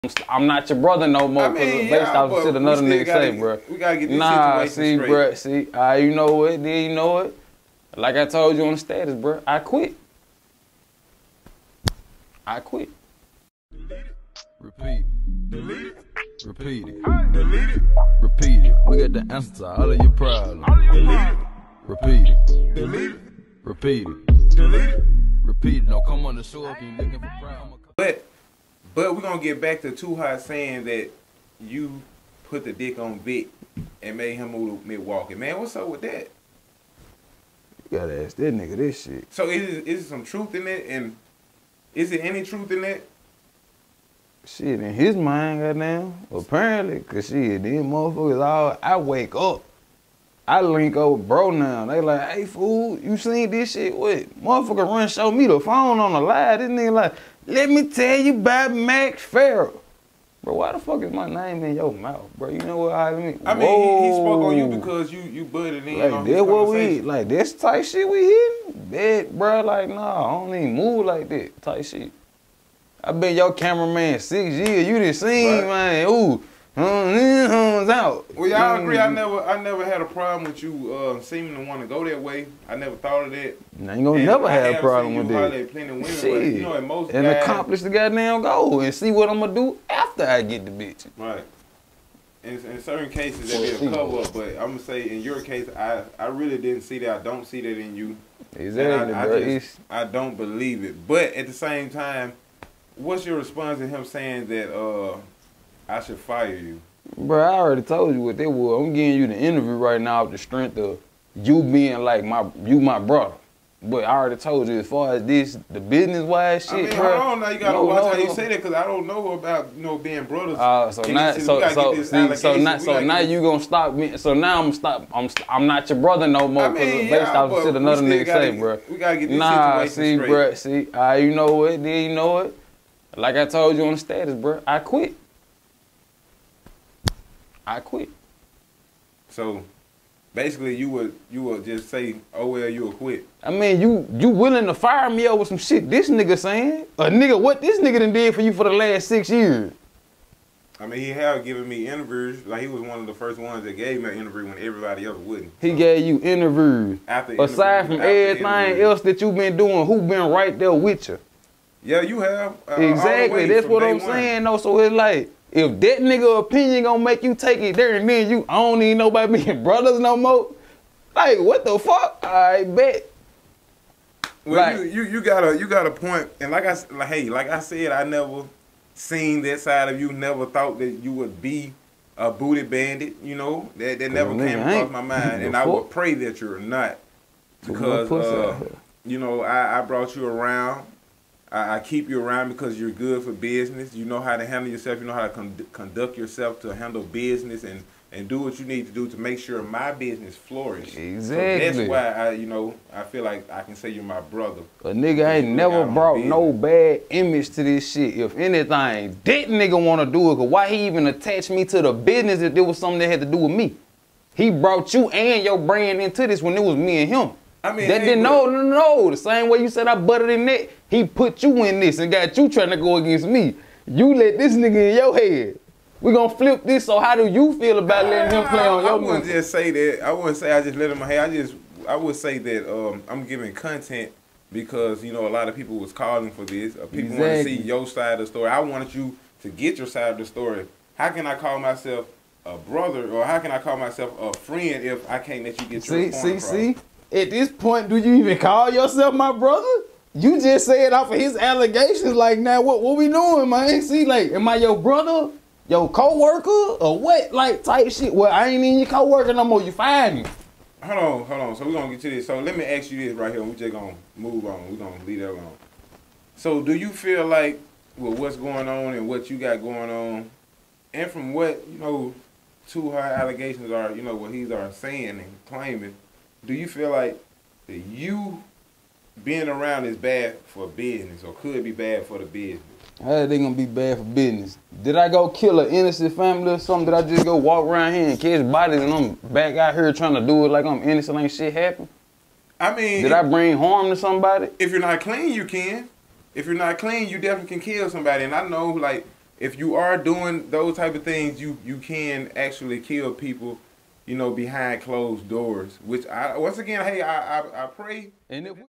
Clip:3 Mac Ferrell speaks gay allegations,being bad for the brand,getting fired,2 Hott & more
Clip:3 Mac Ferrell speaks gay allegations,being bad for the brand,getting fired,Twoo Hott & more